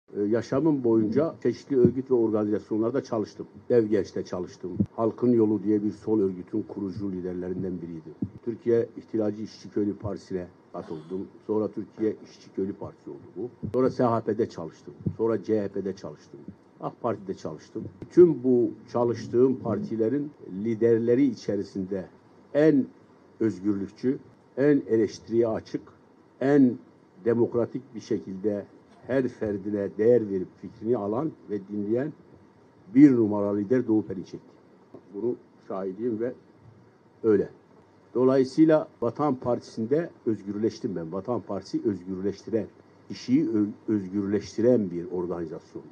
Ethem Sancak: "Vatan Partisi'nde özgürleştim"
Vatan Partisi üyesi iş insanı Ethem Sancak, Vatan Partisi Öncü Gençlik'in düzenlediği eğitimde konuştu. AK Parti'den ayrılmasının hayırlı olduğunu belirten Ethem Sancak, "Vatan Partisi'nde özgürleştim" dedi.